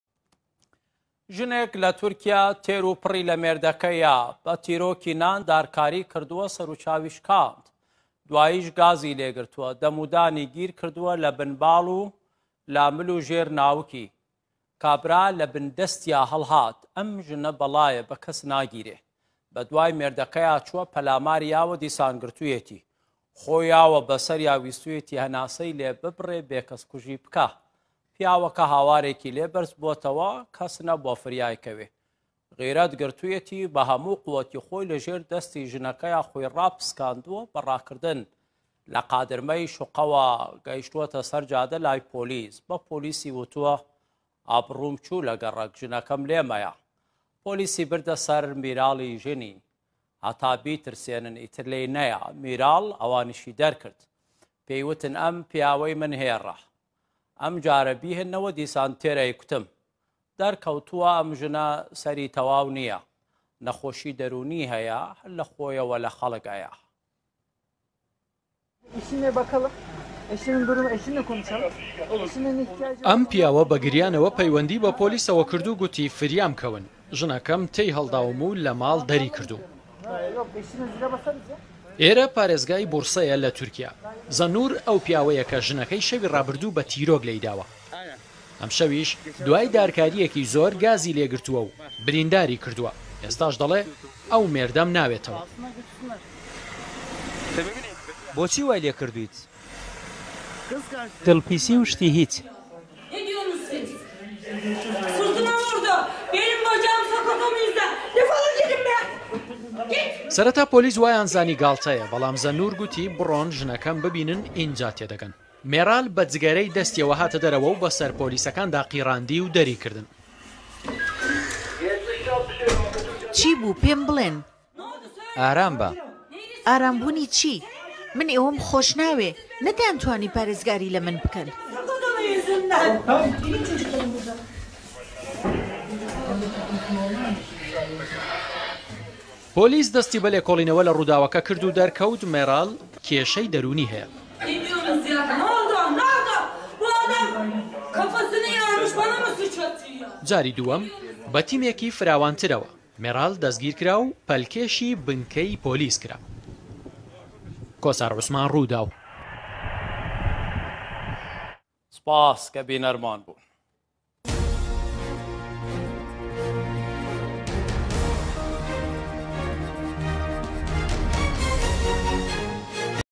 ژنێک مێردەکەی دارکاری کردKocasını oklara ile döven kadın
پیاوێک لە پارێزگای بورسەی تورکیا بە گریانەوە پەیوەندیی بە پۆلیسەوە کرد و گوتی "فریامکەون! ژنەکەم تێیهەڵداوم و لە ماڵ دەریکردووم"، پۆلیس سەرەتا وایزانیوە گاڵتەیە، بەڵام دەرکەوت کە بەڕاستی بووە.
Bursa'nın Osmangazi ilçesi Zinnur B.(41) ile karısı Meral B.(38), kıskançlık sorunu yüzünden tartışmaya başladı. Tartışmanın büyümesiyle Meral B., mutfaktan aldığı oklava ile kocası Zinnur B.’ye saldırdı. Zinnur B.’nin kolunu da ısıran Meral B., başı yarılan kocası Zinnur B.’yi, sokağa çıkma kısıtlamasına karşın, sokağa attı. "O DA BENİM SIRTIMA VURDU" Çaresizce sokakta kalan yaralı Zinnur B., durumu polis ve sağlık ekiplerine bildirdi.

زەننوور ئەو پیاوەیە کە ژنەکەی شەوێک بە تیرۆک لێیداوە، دواتریش دارکارییەکی زۆری کردووە و گازی لێگرتووە و برینداری کردووە، ژنەکە ئێستا دەڵێت ''ئەو مێردەم ناوێتەوە".

پۆلیس لە پیاوەکە دەپرسێت "بۆچی وایلێکردوویت؟" لە وەڵامدا زەننوور دەڵێ، لەسەر "دڵپیسی و شتی هیچ!''

سەرەتا پۆلیس وایانزانیوە قسەکانی زەننوور گاڵتەیە، بەڵام زەننوور پێی گوتوون، بڕۆن ژنەکەم ببینن ئینجا تێدەگەن. کە پۆلیس چووەتە ماڵەکەی، سەرەتا مێراڵ-ی هاوژینی زەننوور بە جگەرەیەکەوە بەسەر پۆلیسەکاندا هاواری کردووە و دەریکردوون.

مێراڵ-ی هاوژینی زەننوور بە پۆلیسەکانی گوتووە "چی بوو پێمبڵێن؟ ئارامبوونی چی؟ من ئێوەم خۆشناوێت، نەتانتوانی پارێزگاری لە من بکەن".

پۆلیس دەستی بە لێکۆڵینەوە لە رووداوەکە کردووە و دەرکەوتووە مێراڵ کێشەی دەروونیی هەیە. جاری دووەم پۆلیس بە تیمێکی فراوانترەوە مێراڵی پەلکێشی بنکە کرد و گوتەیان لێوەرگرت.